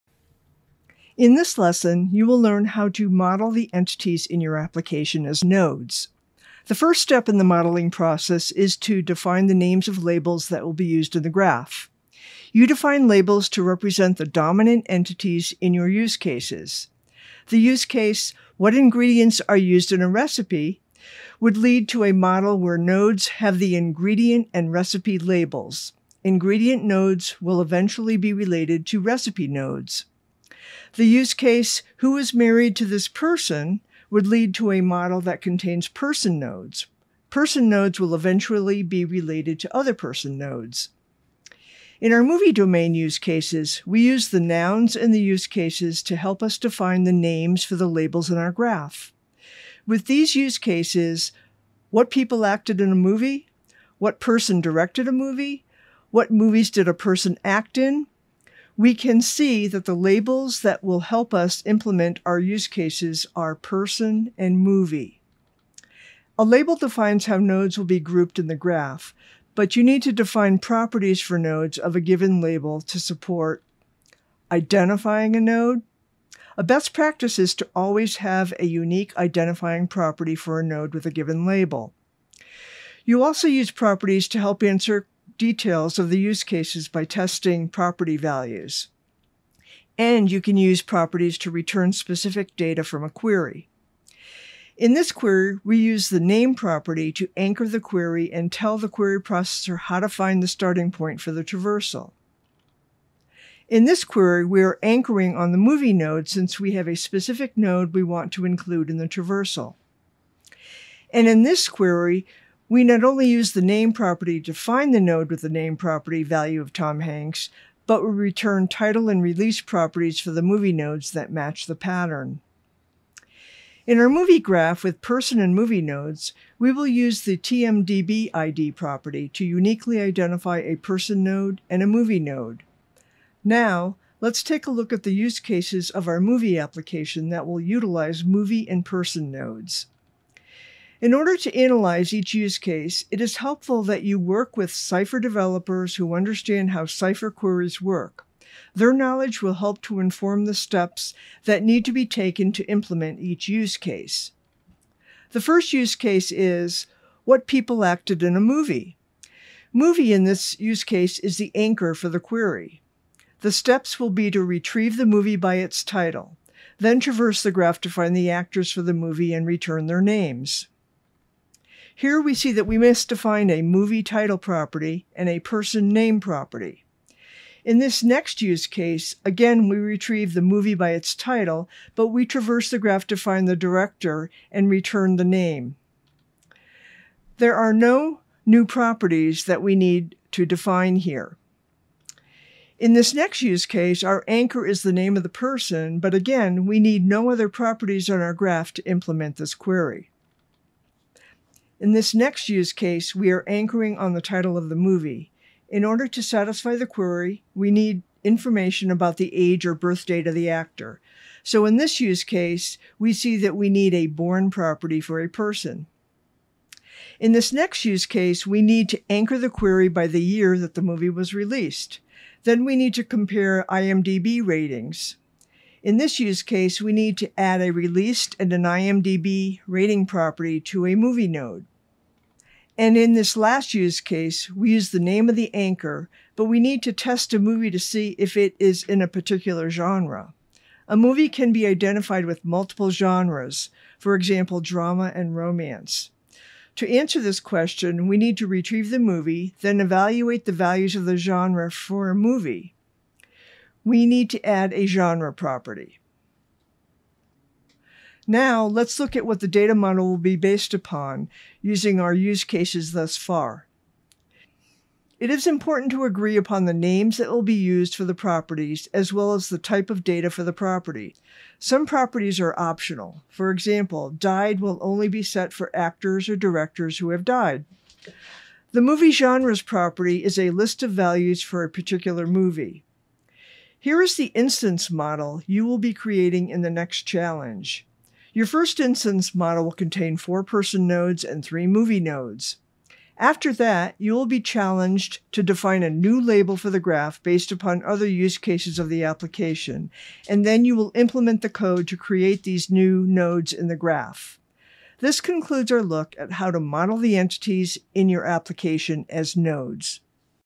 Graph Modeling Fundamentals - Modeling Nodes - Modeling Nodes
Learn how to model the entities in your application as nodes.